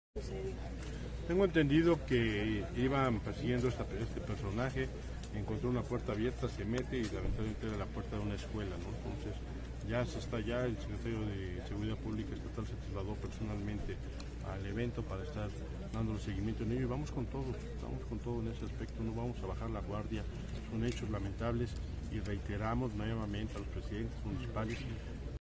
El gobernador de Puebla exhortó a los ediles municipales a enviar seguridad a las centros educativos
El gobernador de Puebla, Sergio Salomon Céspedes, llamó a los presidentes municipales a continuar enviando seguridad a las escuelas luego de los hechos ocurridos este jueves en Moyotzingo. La maestra lesionada está estable y el hombre fallecido podría estar relacionado con actividades ilícitas.